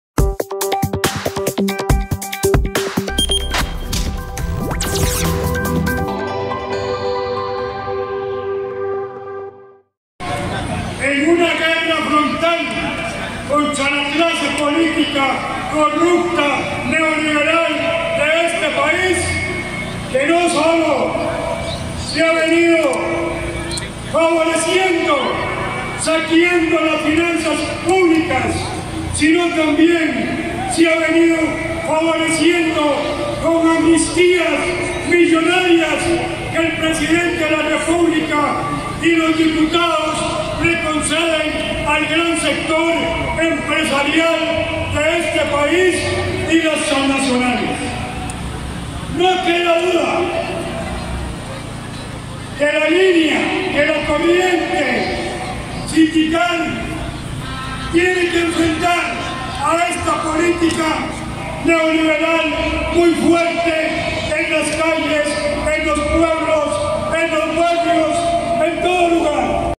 ACUSACIONES UNDECA
El líder sindical de UNDECA, Luis Chavarría lanzó duras acusaciones contra el Gobierno en la marcha de los sindicatos del sector salud